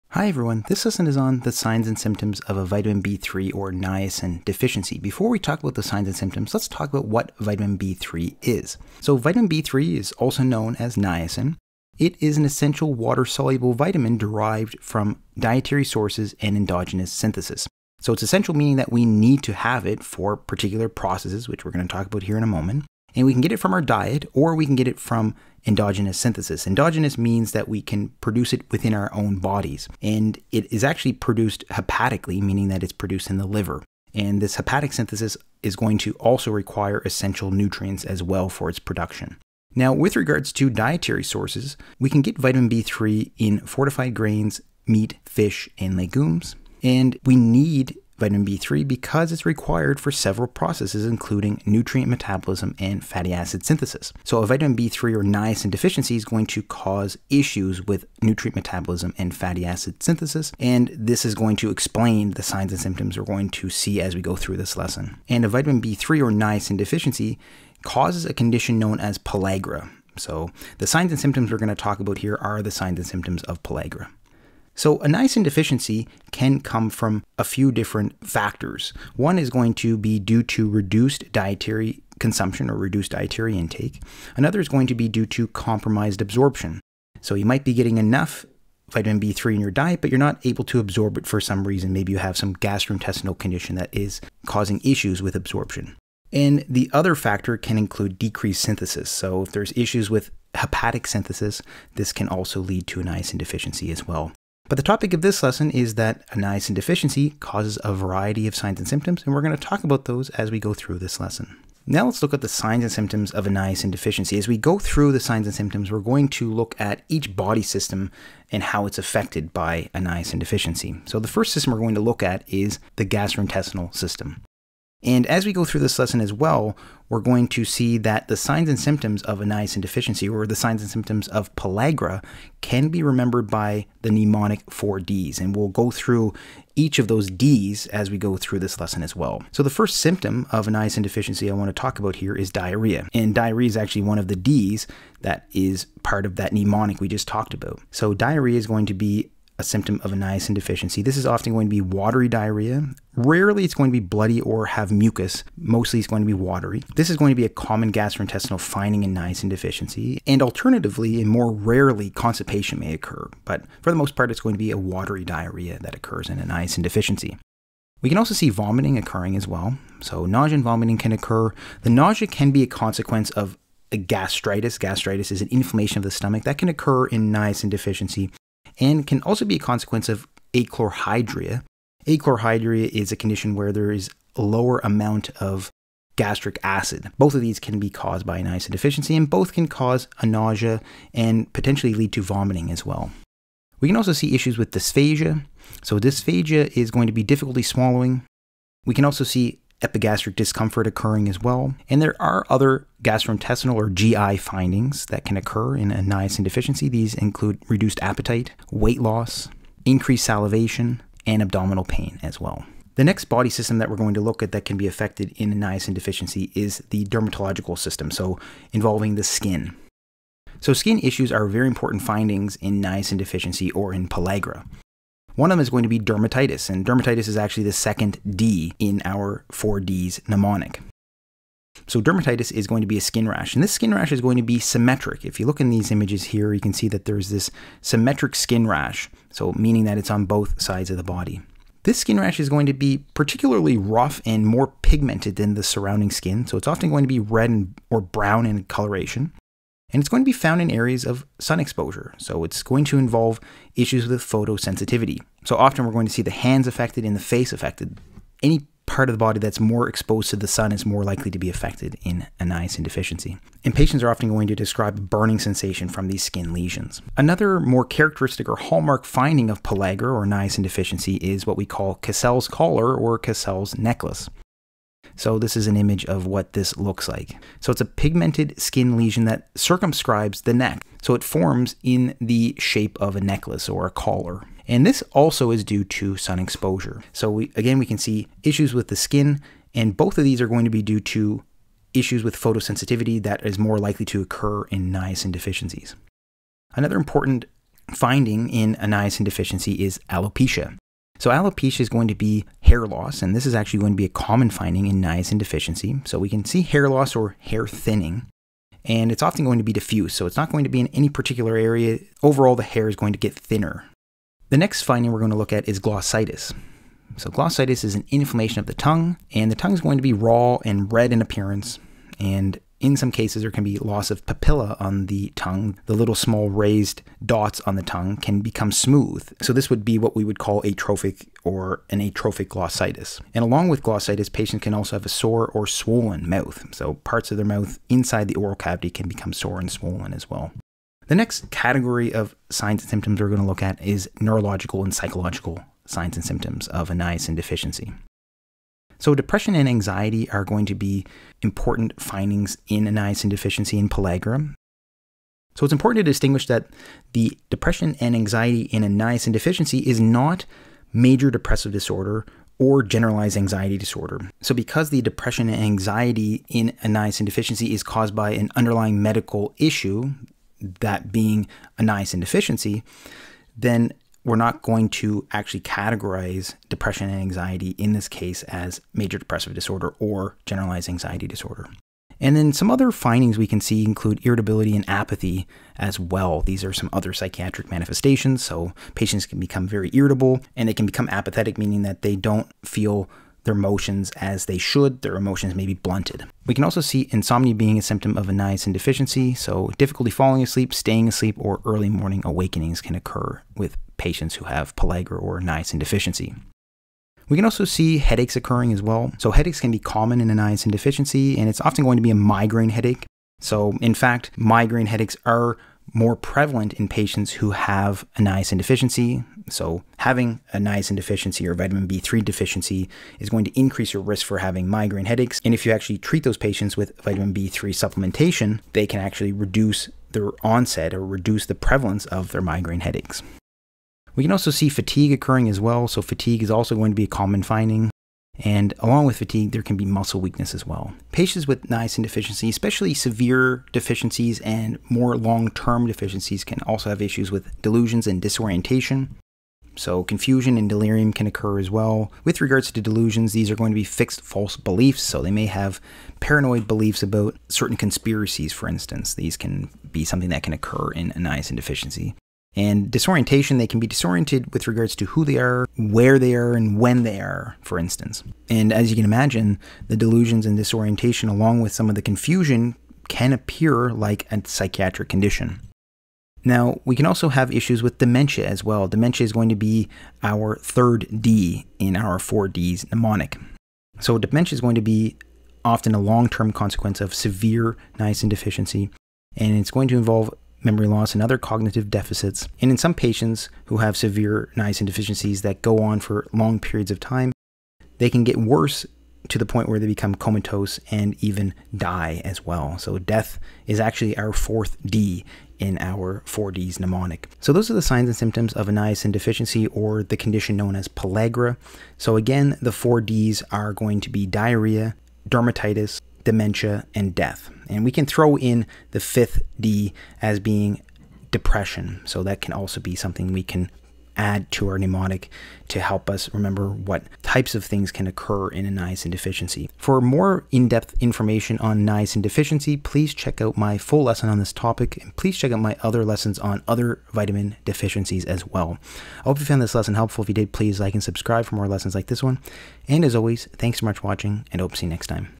Vitamin B3 (Niacin) Deficiency Signs & Symptoms (Skin, Hair, Gastrointestinal, Psychiatric)
Vitamin B3 (also called Niacin) is an essential, water-soluble vitamin that is required for several important biological processes including nutrient metabolism and fatty acid synthesis. A vitamin B3 deficiency (which is also called Pellagra) can cause the condition known as pellagra and can cause a wide variety of gastrointestinal, dermatological and psychiatric symptoms, which we discuss in this lesson.

JJ

For books and more information on these topics